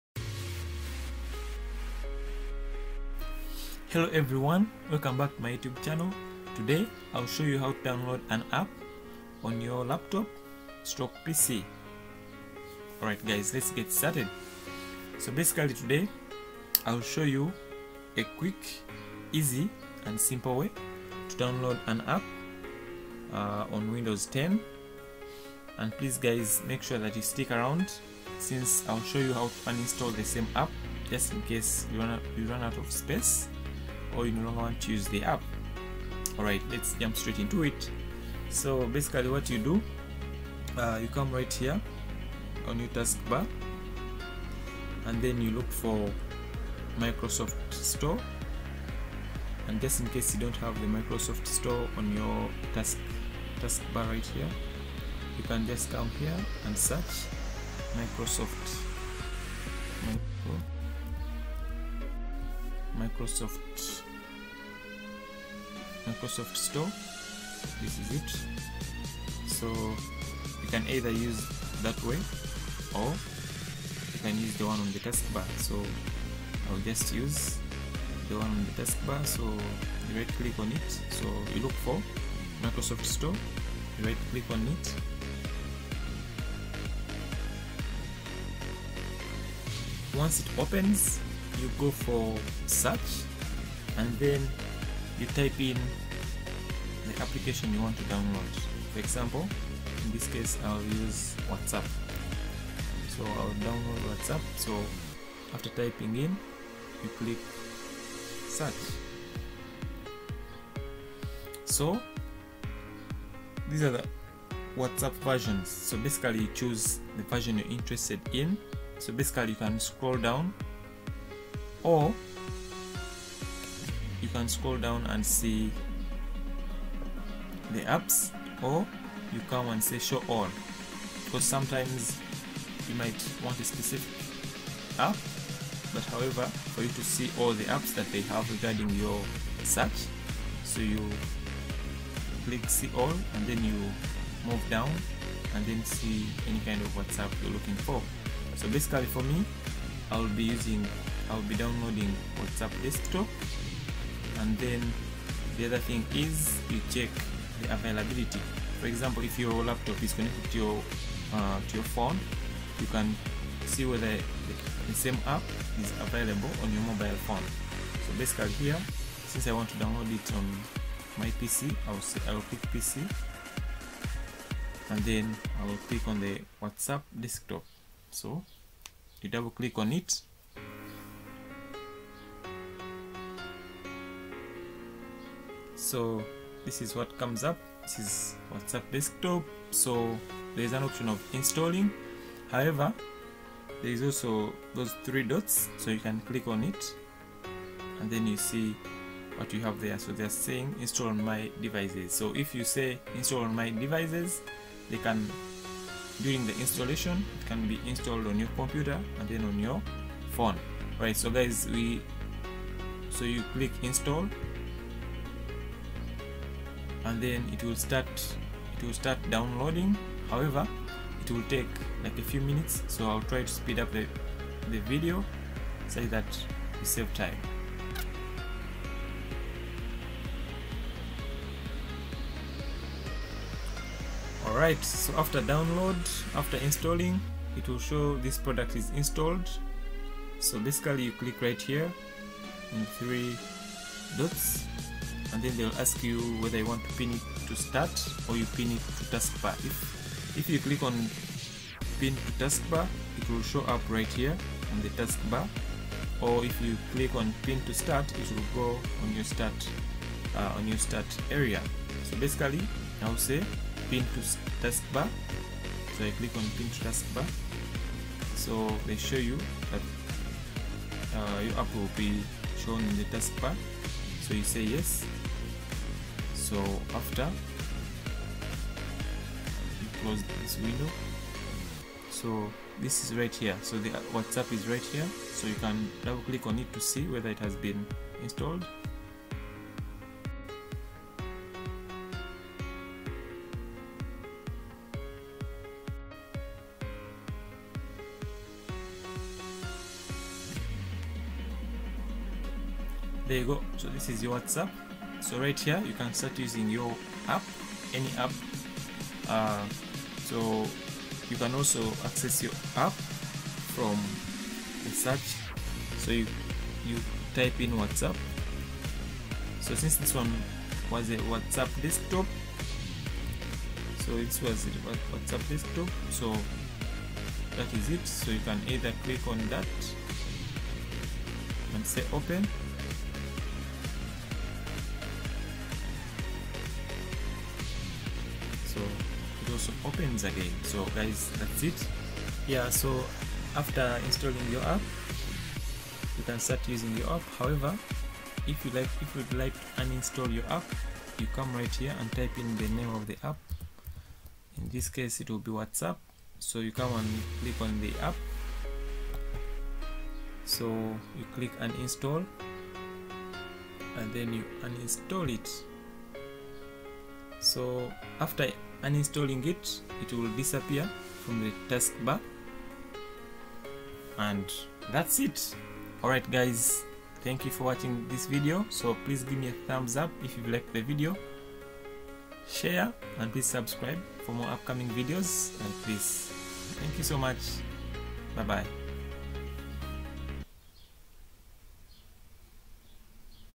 Download any APP in windows 10 | How to Download and Install any APP in windows 10 for free
In this video, You will learn how to download and install any APP in windows 10 on your PC/Laptop using Microsoft store(Windows store).